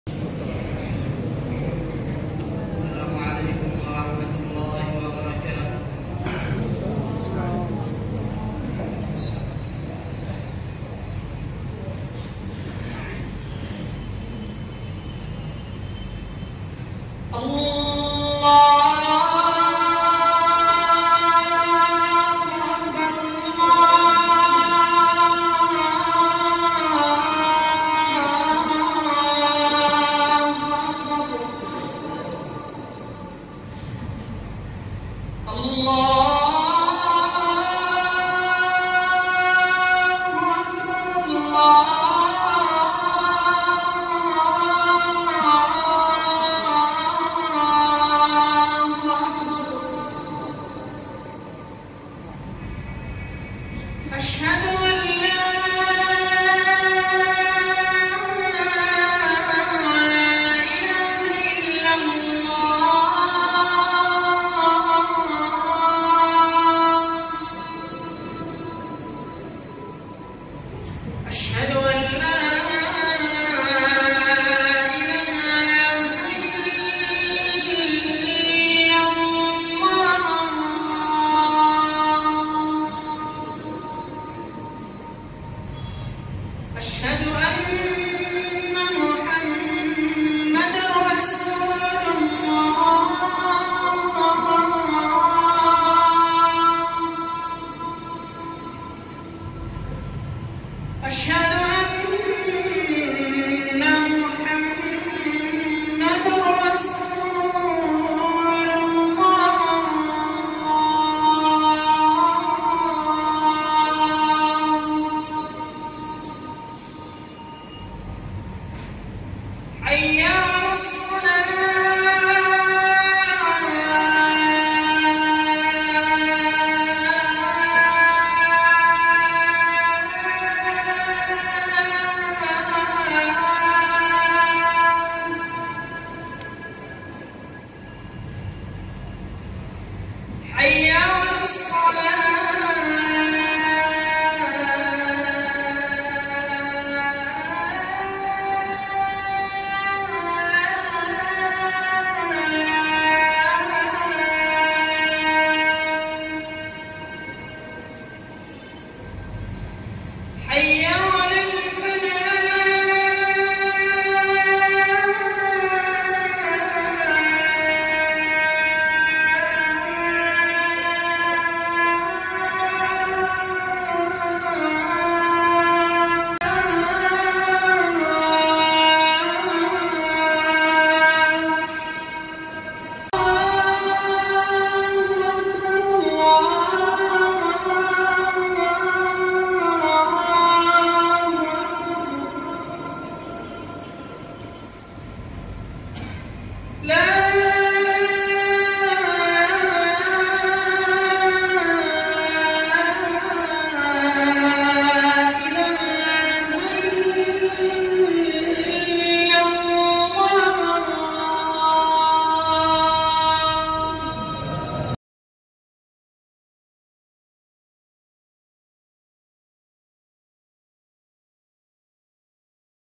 أذان صلاة الجمعة بجامع المصري في مدينة مأرب 20 مارس 2020 م اليمن
أذان صلاة الجمعة بجامع المصري في مدينة مأرب اليوم 20 مارس 2020 م و ذلك بعد قرار العديد من الدول بمنع إقامة الصلاة بالمساجد بسبب فيروس كورونا .
20 مارس 2020

مارب
اليمن

Abdrhman Almasri

Yemen

Marib

لمشاهدة مقاطع فيديو بقناتي باليوتيوب اضغط على الرابط التالي